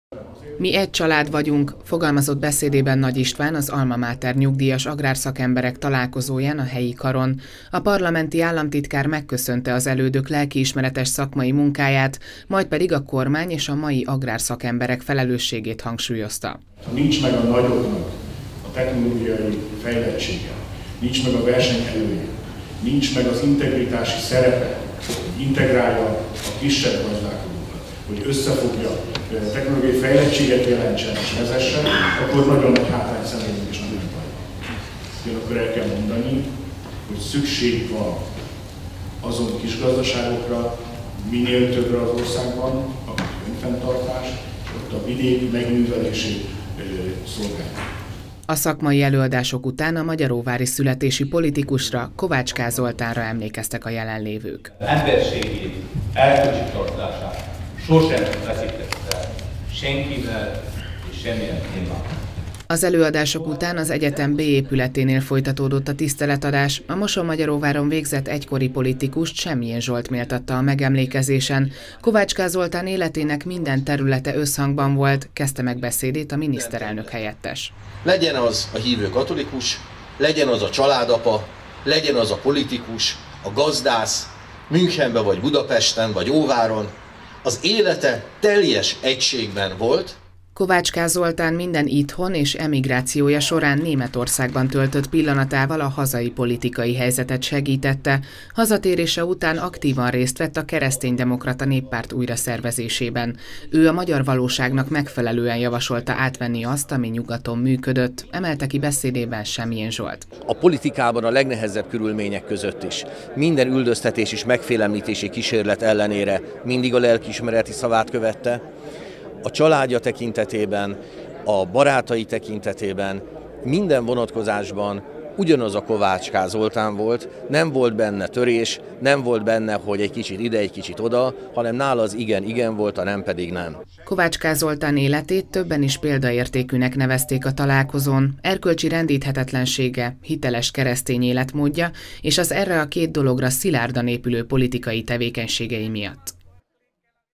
Nyugdíjas Agrárszakemberek Találkozója
Ma délelőtt tartották az Alma Mater Nyugdíjas Agrárszakemberek Találkozóját a helyi karon. Az előadások után a vendégek felavatták a Kovács K. Zoltán tiszteletére állított emléktáblát. Az egykori országgyűlési képviselő élete során aktívan részt vett a mosonmagyaróvári és környékbeli közéletben.